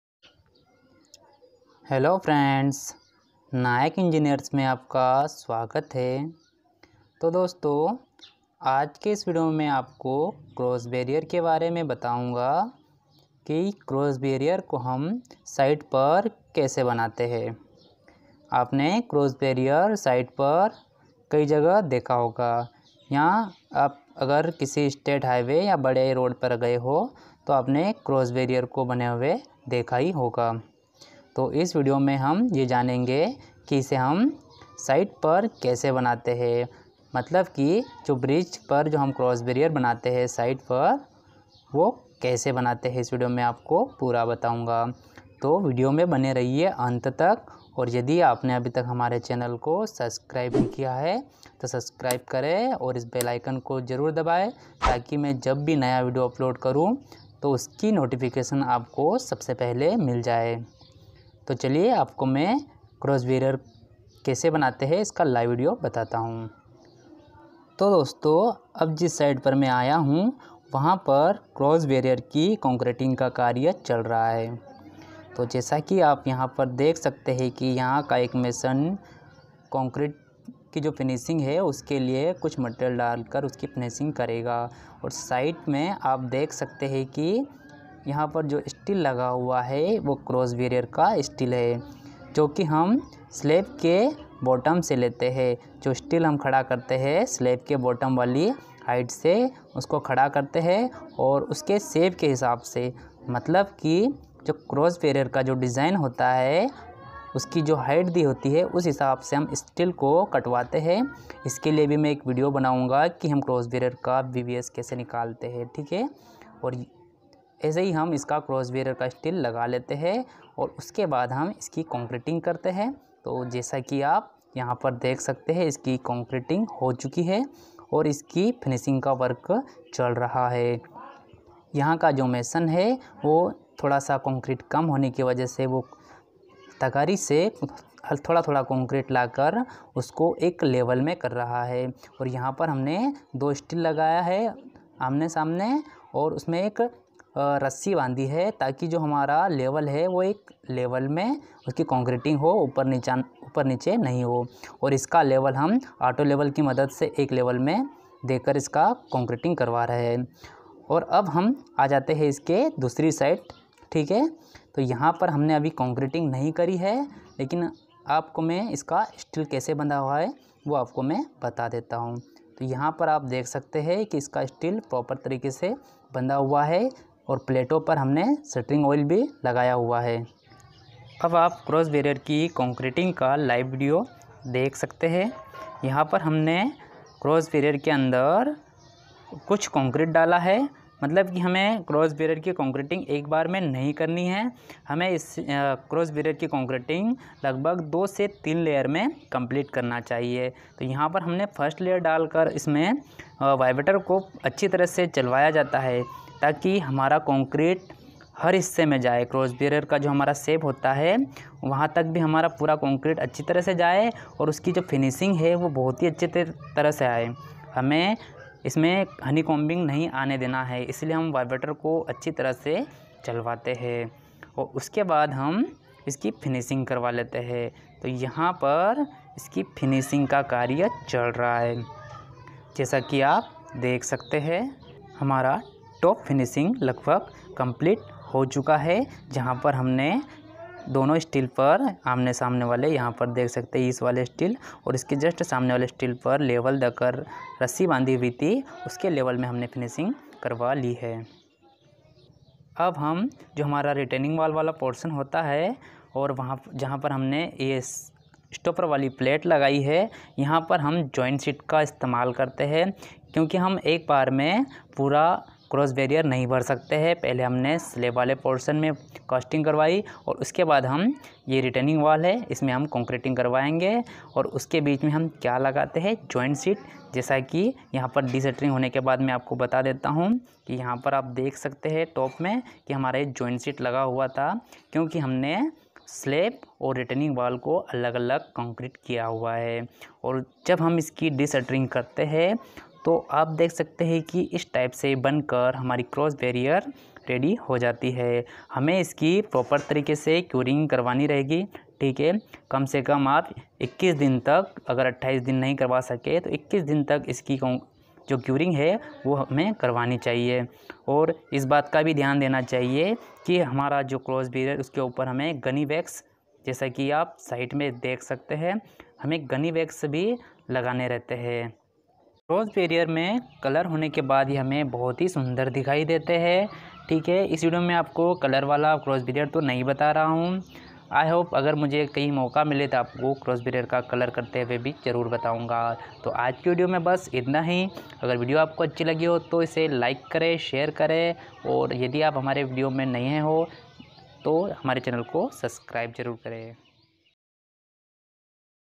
Crash Barrier// crash Barrier making Live video
Hello friends
in this video show the crash Barrier making Live video at site.